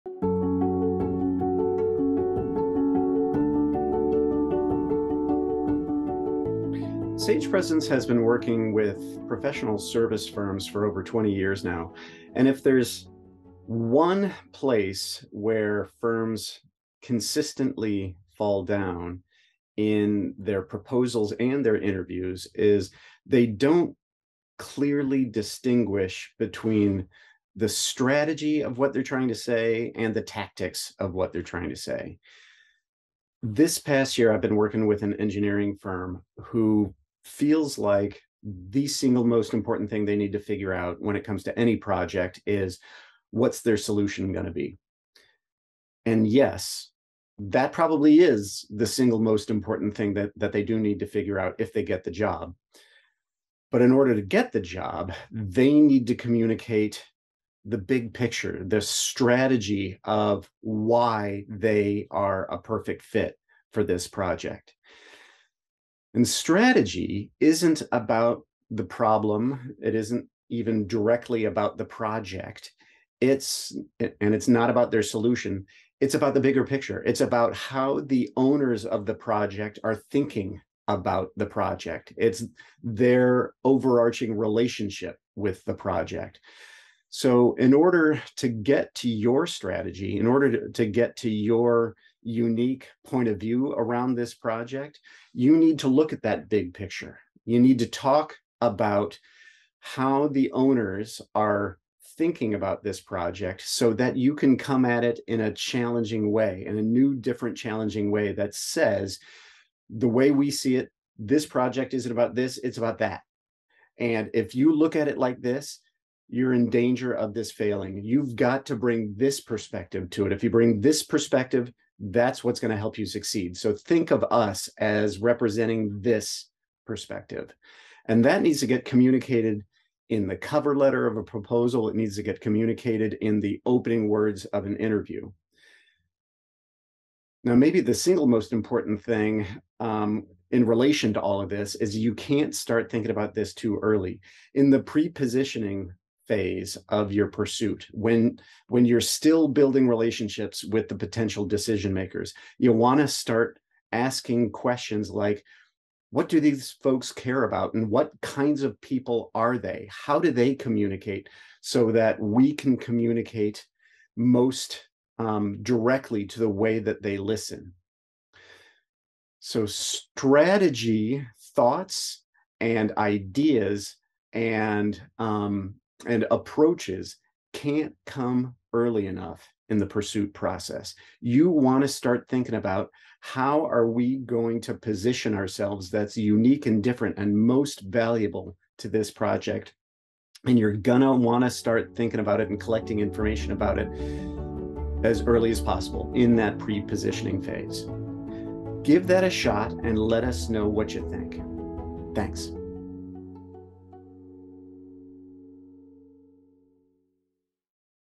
Strategy Begins Early - Pete Machalek
All too often, technically-minded service providers think that the one and only thing they need to communicate about is their solution to a prospective problem. This is understandable because problem-solvers zero in on problems and love to solve them. But it's a critical mistake because it fails to take the bigger picture into consideration. It leaves out an understanding of how the prospect is relating to their problem. Ultimately, it's all about tactics and nothing about strategy, and it massively lessens your ability to win a project.

In today's vlog, Pete talks about how to shift this so that every team member starts thinking about strategy early in the process and hangs onto that mindset throughout the whole pursuit. Check it out and let us know your thoughts!

Check out our philosophy and let us know what you think!